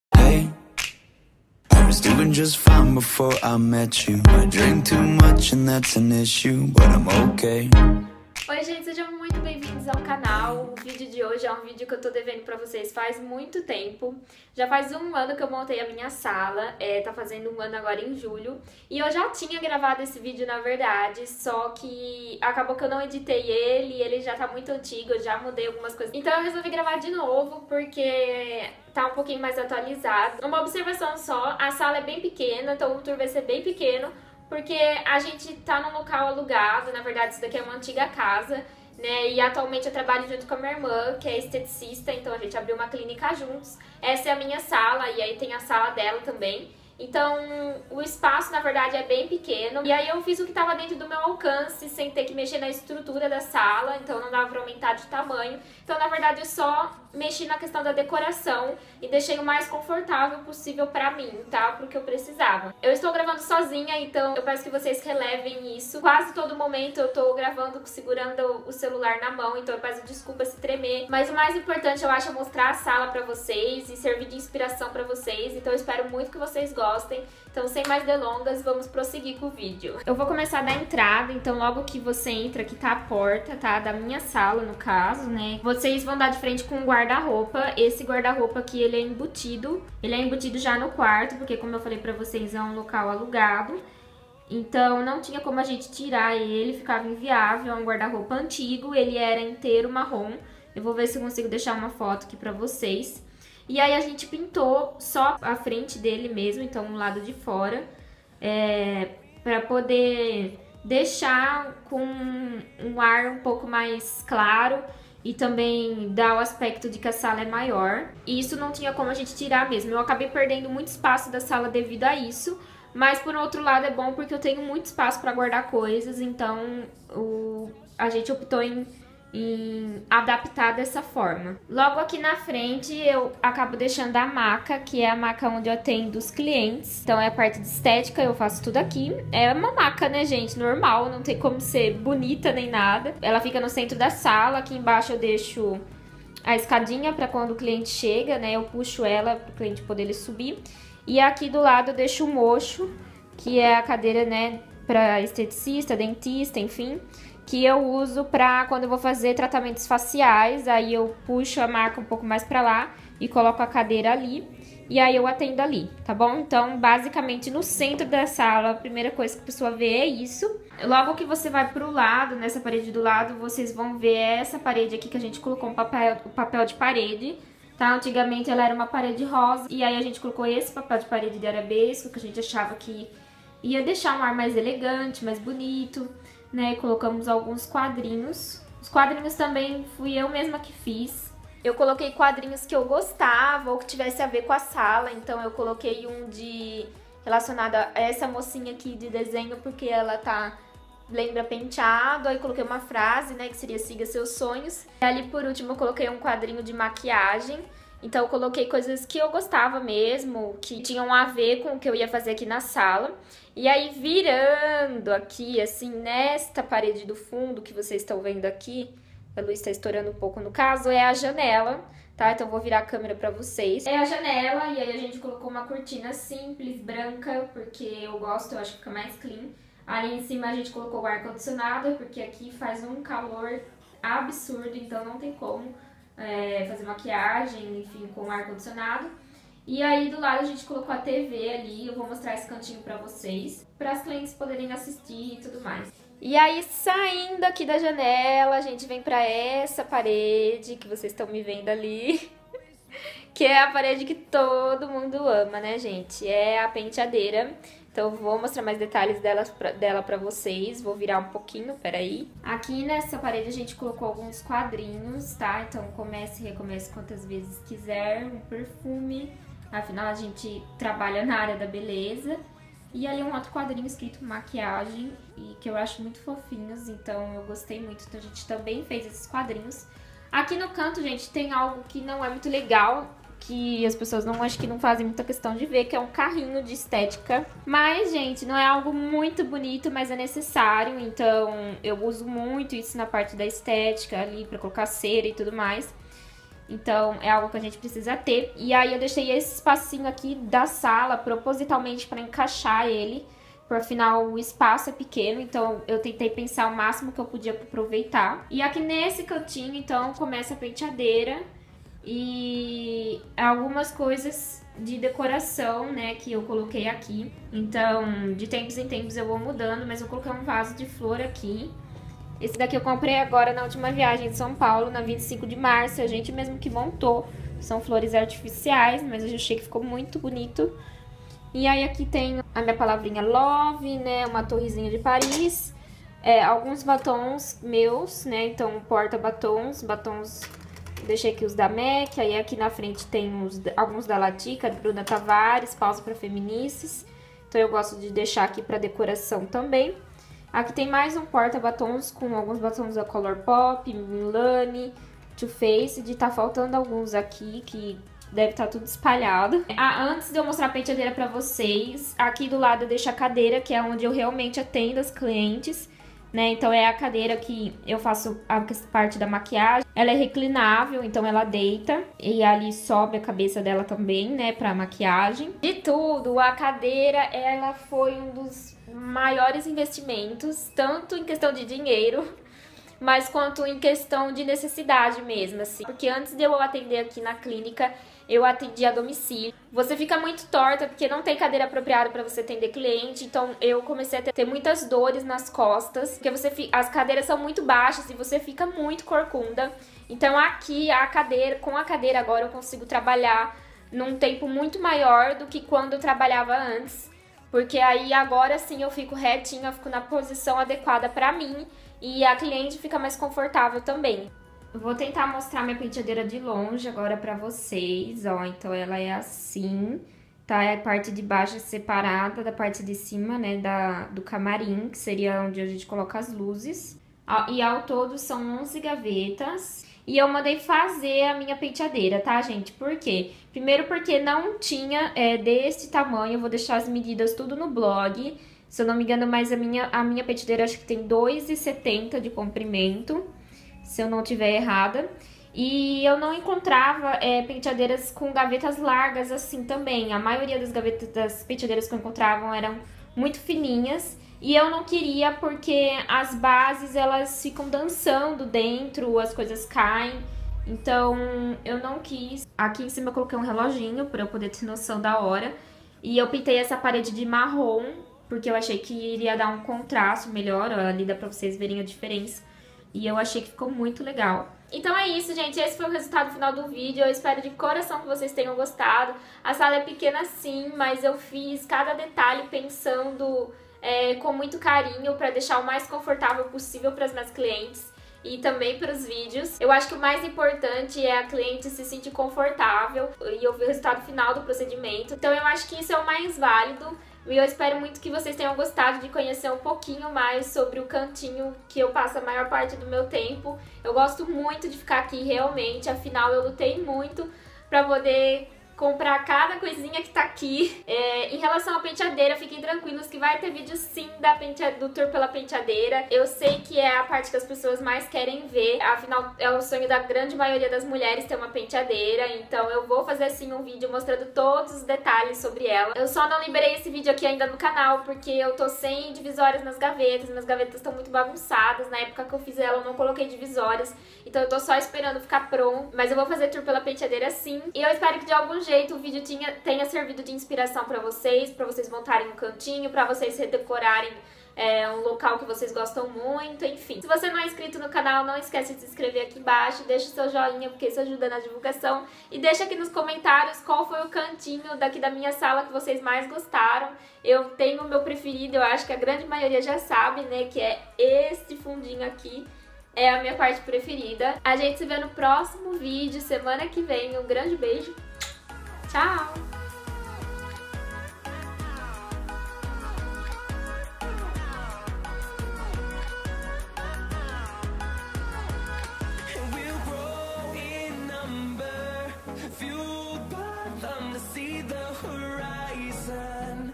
Tour pelo meu Ateliê de Maquiagem e Estética
O vídeo de hoje foi um dos vídeos mais pedidos aqui no canal!! Já faz 1 ano que eu tenho minha sala de atendimentos onde realizo todos os procedimentos de maquiagem e estética porém nunca havia conseguido trazer aqui pro canal todos os detalhes da sala onde foi tudo pensado e planejado.
Agora sim com todos os detalhes prontos apresento pra vocês o cantinho onde eu passo a maior parte do tempo e onde tudo acontece!
Espero de coração que vocês gostem e que sirva de inspiração!!

Nos vemos no próximo vídeo! (Toda quarta-feira às 20:00)

Um grande beijooo até a próxima!!